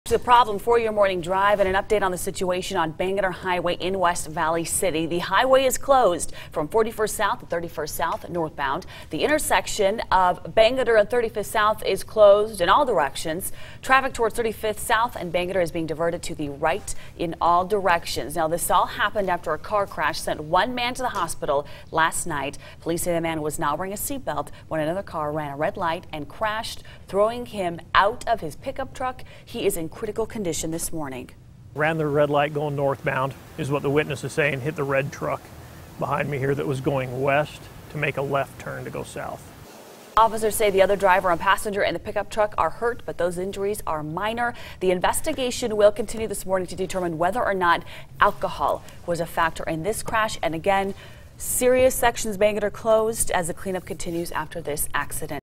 Car accident leaves one man in critical, shuts down Bangerter Highway
WEST VALLEY CITY, Utah (ABC 4 UTAH) - A man is in critical condition after a car accident on Bangerter Highway and 3500 South. The accident happened around 11:30 PM Monday night and closed the intersection for 7 hours.

Police say the man wasn't wearing a seatbelt when another car ran a red light and crashed, throwing him from his pickup truck.

The two people in the other car suffered minor injuries in the accident. Police say they are investigating to see whether or not alcohol was a factor in the crash.